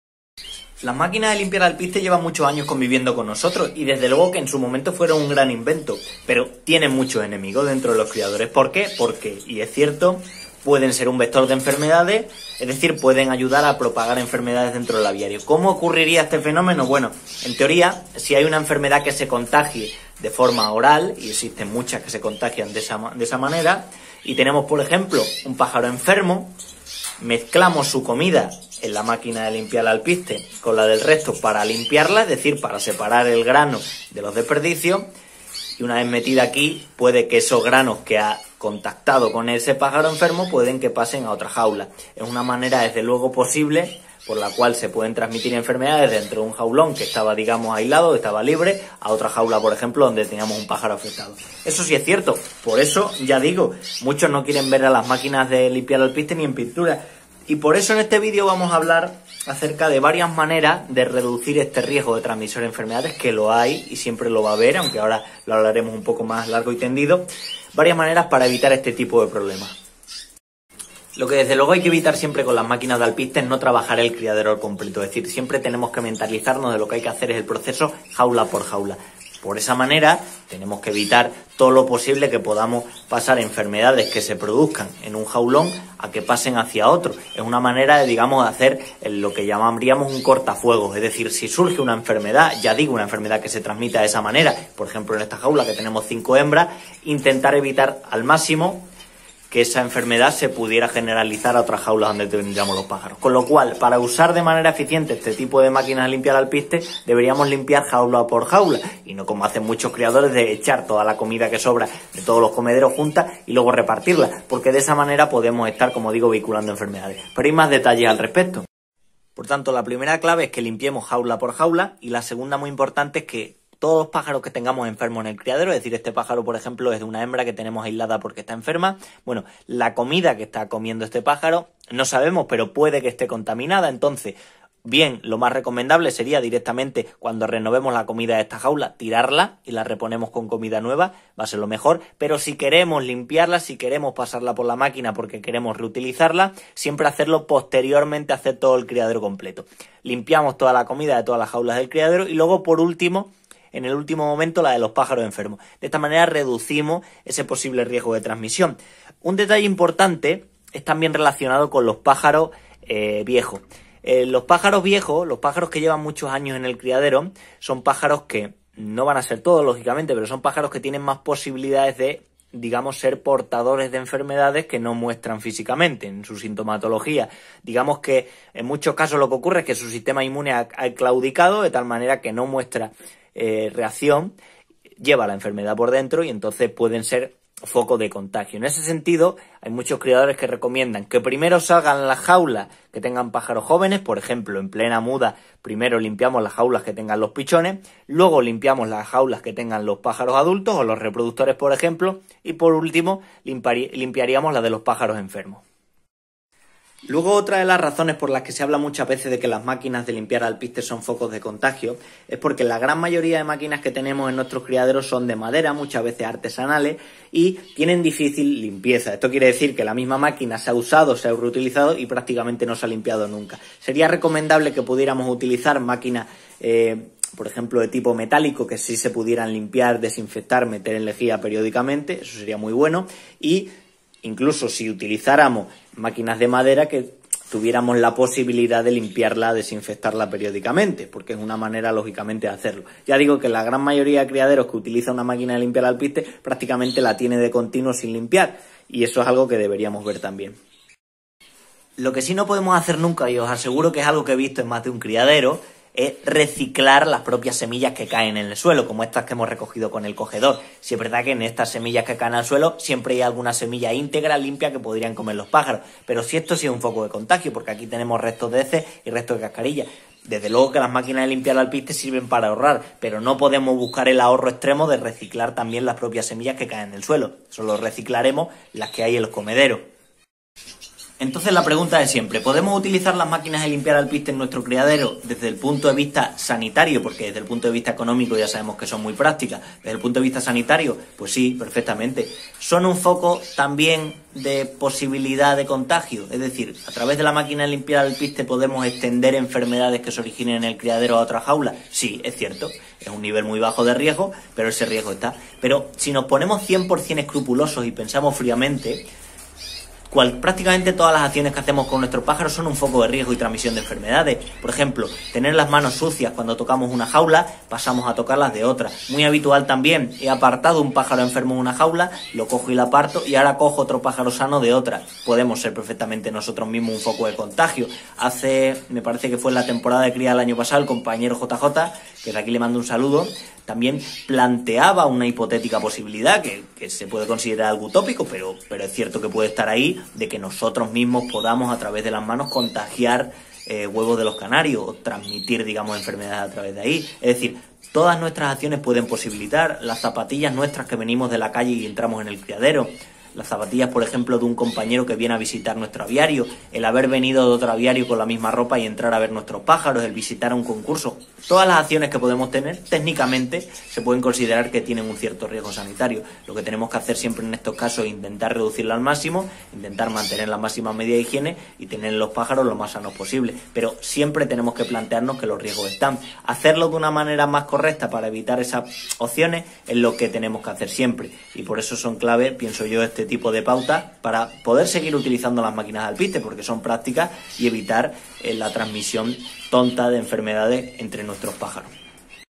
SOLUCIONES A MAQUINAS DE LIMPIAR ALPISTE: Vector de enfermedades.
Reflexionamos en el video por que las maquinas de limpiar alpiste se considerar un posible transmisor de enfermedades, hasta que punto es cierto y como podemos reducir este riesgo.